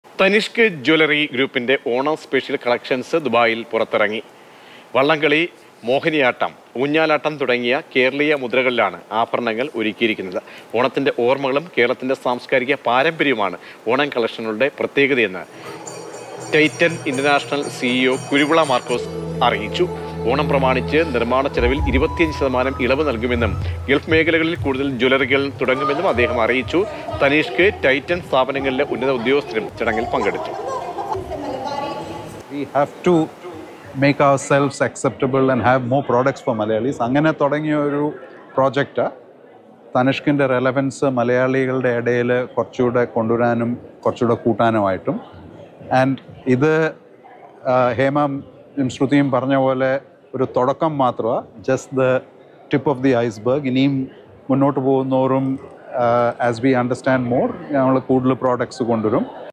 ഓണം സ്പെഷ്യൽ കളക്ഷൻസ് പുറത്തിറക്കി തനിഷ്ക് ജ്വല്ലറി ഗ്രൂപ്പ് | Tanishq
ഓണം സ്പെഷ്യൽ കളക്ഷൻസ് പുറത്തിറക്കി തനിഷ്ക് ജ്വല്ലറി ഗ്രൂപ്പ്

മാറുന്ന ലോകത്ത് കൂടുതൽ മാറ്റങ്ങളോടെ. പുതിയ വേഗത്തിൽ. പുതിയ ലുക്കിൽ.
മാതൃഭൂമി ന്യൂസ്.

Happy viewing!